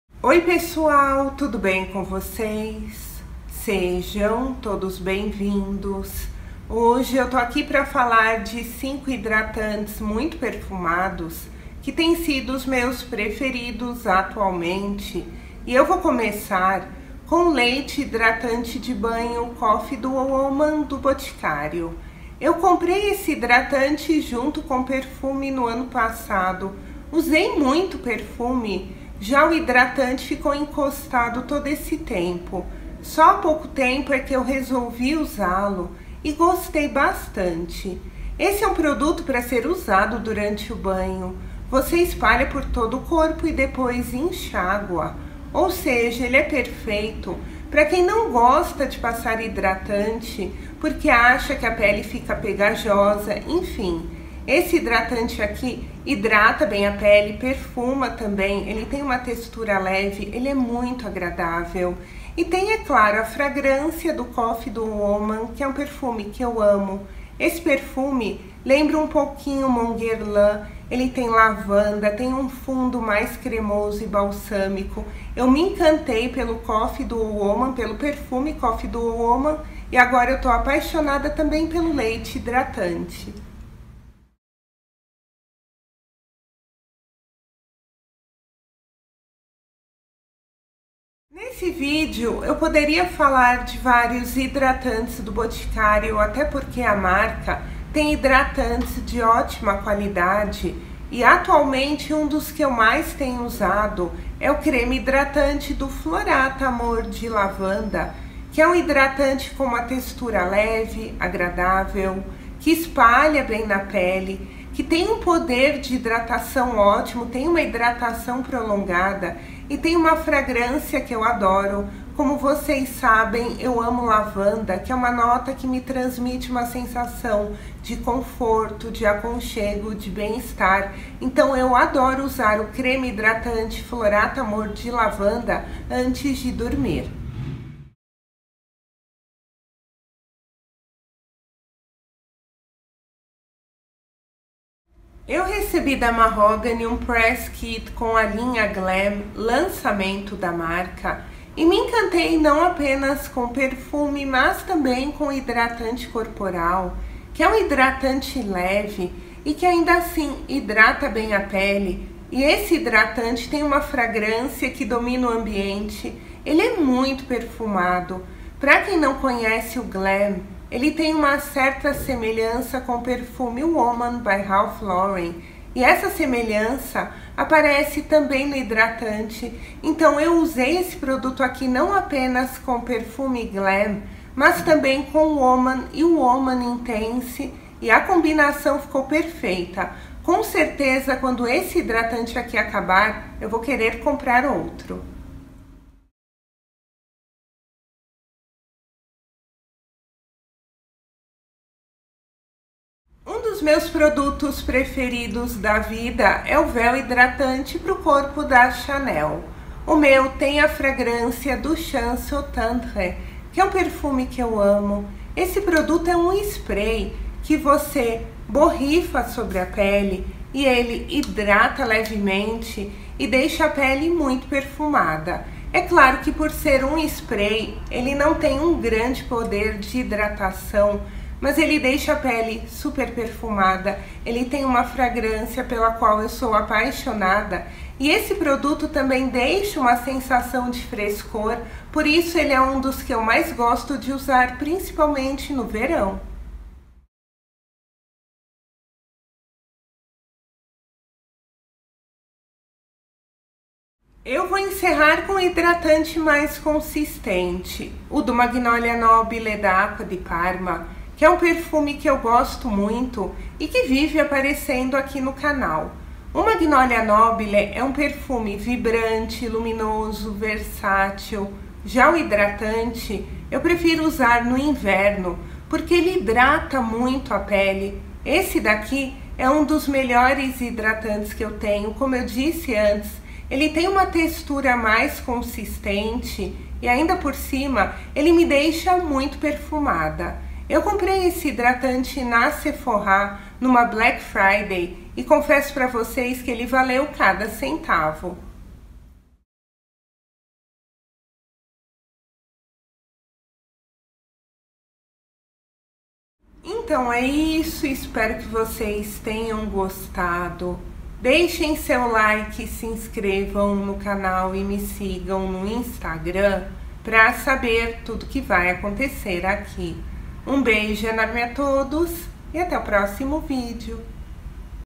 Top 5 Hidratantes Que Substituem Perfumes (Favoritos do Momento)
Produtos apresentados:

Leite Hidratante de Banho Coffee Duo Woman (o Boticario)
Creme Hidratante Floratta Amor de Lavanda (o Boticario)
Hidratante Glam (Mahogany)
Véu Hidratante para o Corpo Chance Eau Tendre (Chanel)
Creme Hidratante Corporal Magnolia Nobile (Acqua di Parma)

❤️Para comprar produtos Natura, acessem o espaço da Consultora Regina Porto